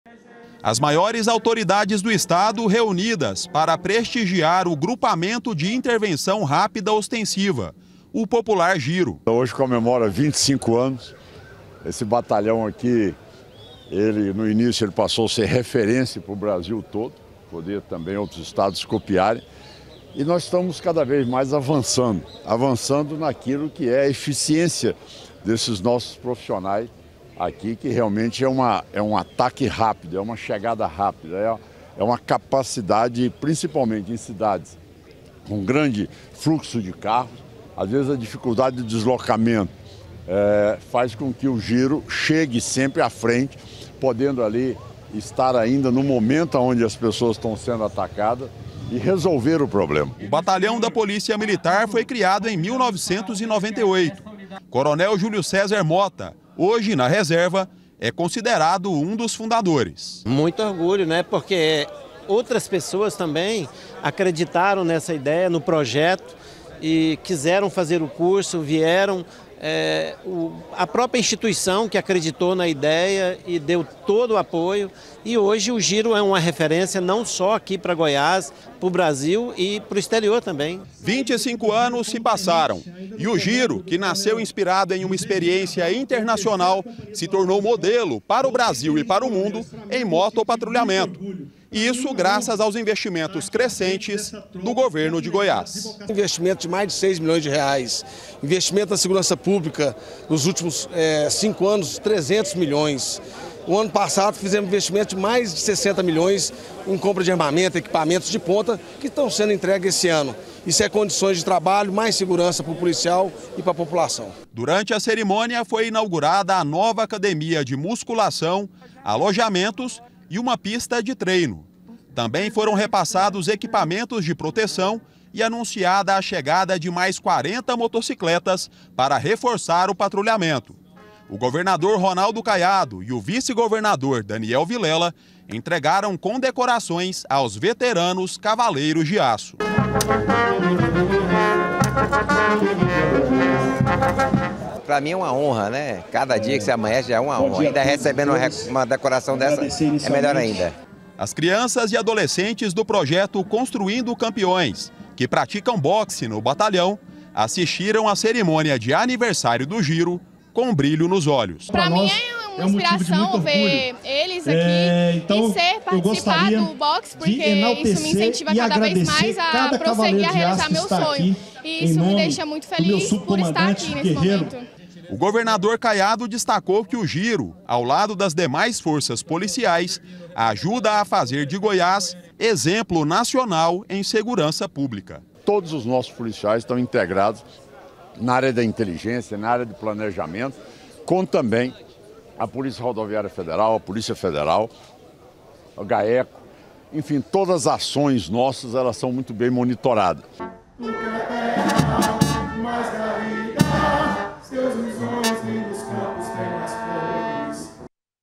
REFERÊNCIA NACIONAL: GIRO DA POLÍCIA MILITAR COMPLETA 25 ANOS
40 NOVAS MOTOCICLETAS VÃO REFORÇAR O PATRULHAMENTO DA EQUIPE DO GIRO DA POLÍCIA MILITAR. O ANÚNCIO FOI FEITO HOJE DURANTE O ANIVERSÁRIO DO BATALHÃO.